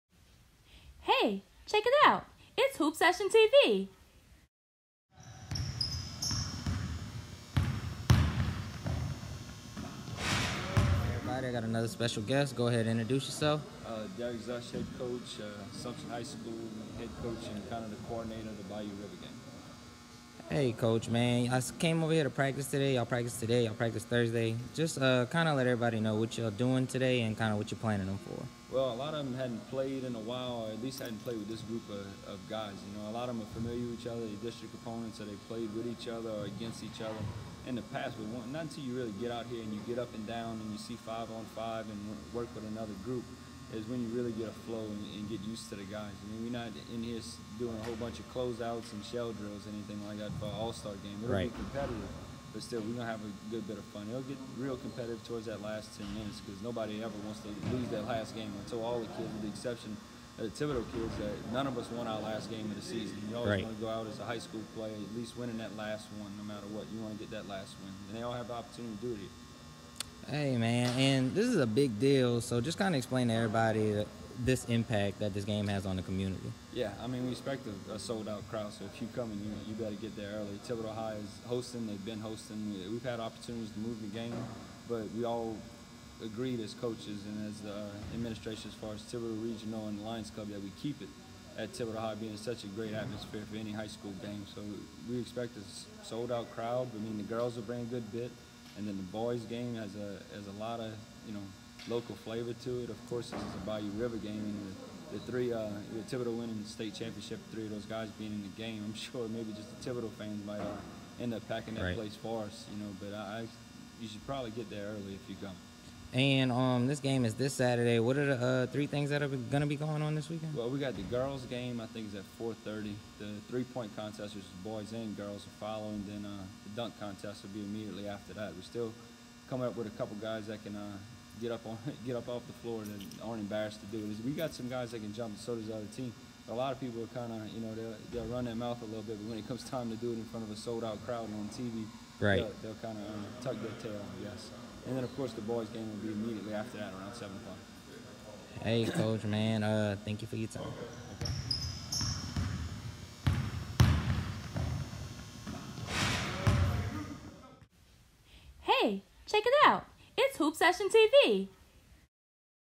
Hoop Session TV: Bayou/River West Allstar practice interview with Coach Szush
Hoop Session TV: West Allstar practice interview with Coach Szush. This Saturday March 23,2019 will be the Bayou/River Shootout. Everyone come on and support!